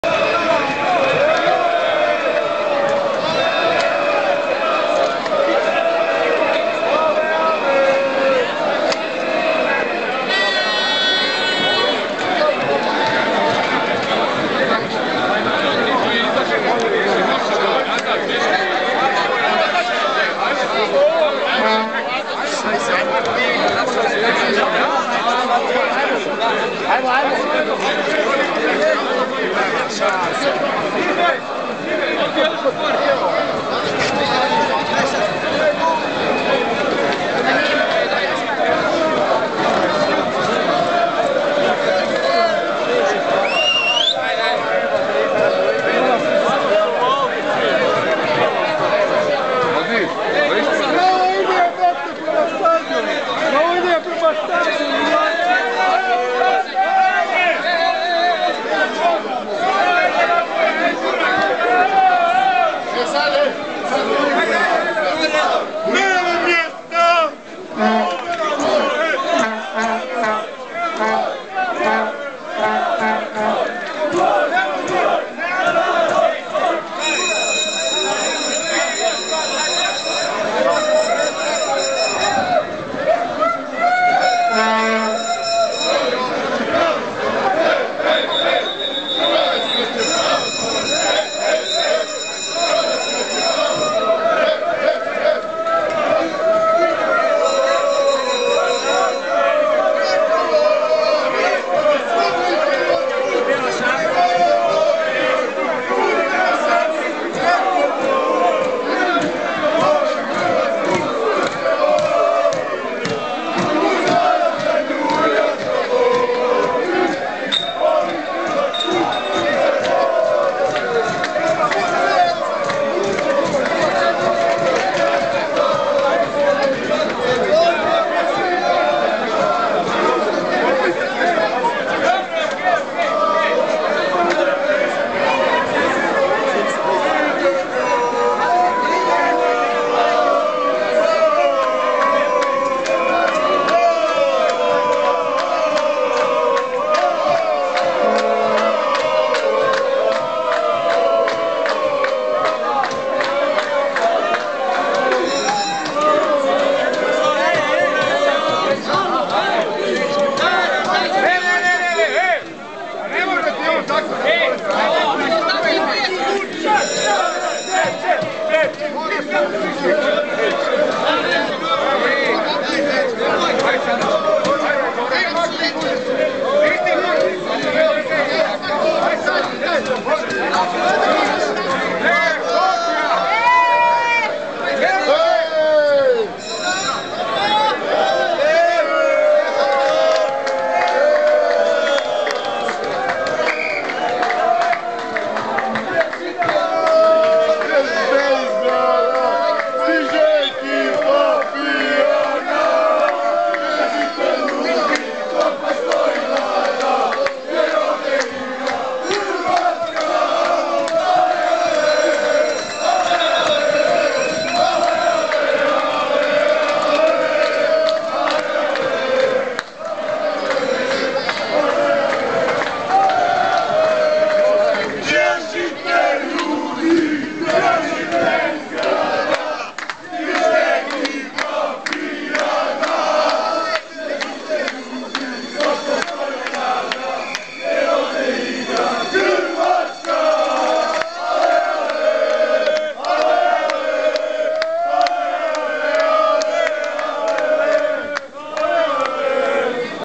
(3) EURO 2012, Chorwacja - Włochy - droga na stadion / CRO vs. ITA, fans (2012.06.14)
Chorwaccy, Włoscy i Polscy kibice w drodze na mecz Chorwacja - Włochy podczas EURO 2012 (Polska - Ukraina). Komunikacja miejska w Poznaniu. Stare Miasto, okolice Placu Wolności i strefy kibica (Fan Zone).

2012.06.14

Witamy w Poznaniu, powodzenia:)

Croatian, Italian and Polish football fans at the Public Transport of Poznan (trams & buses). Croatia vs. Italy match day, en route to Stadium. UEFA EURO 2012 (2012 UEFA European Football Championship). Old City, near official Fan Zone.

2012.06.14

Welcome to Poznan, good luck:)

Polska / Poland

Republika Hrvatska / Chorwacja / Croatia

Italia / Włochy / Italy